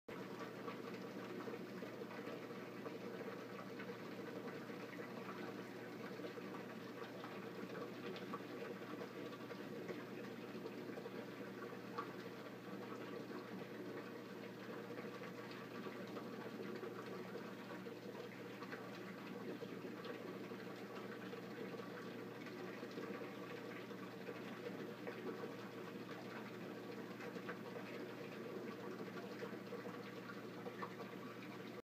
Spike n Oscar
Pike Blenny, Jawfish and Anemone. Reef tank.